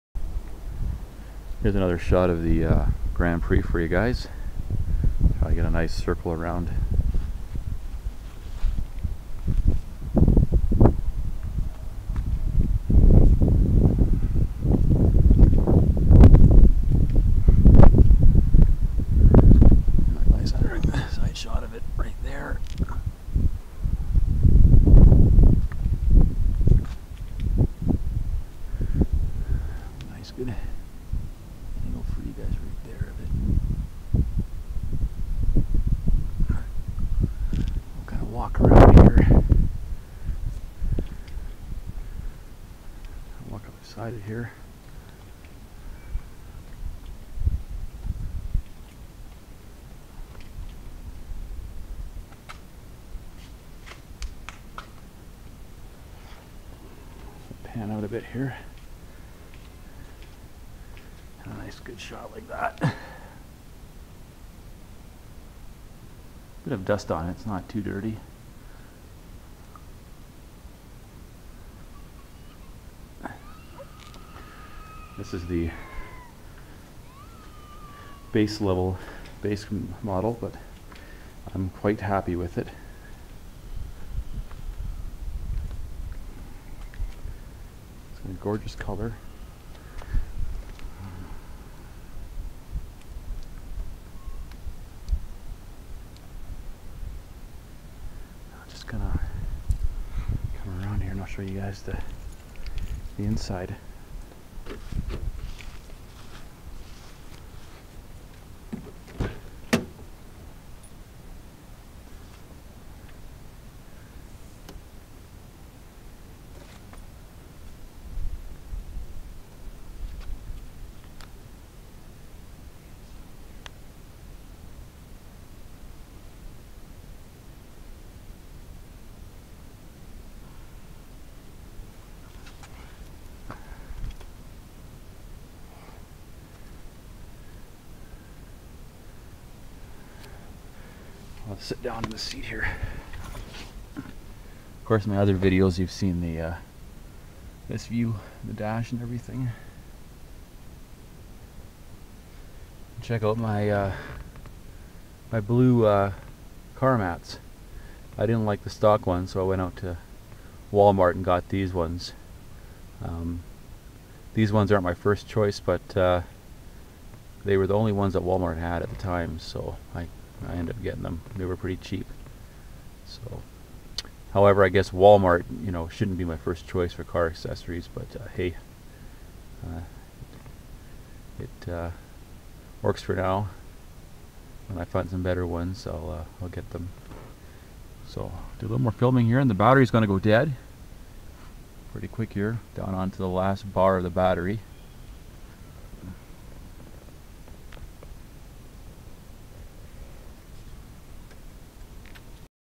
Walk around of my Grand Prix
Just a quick shot of my 2008 Grand Prix. Wanna know how to get a beauty like this?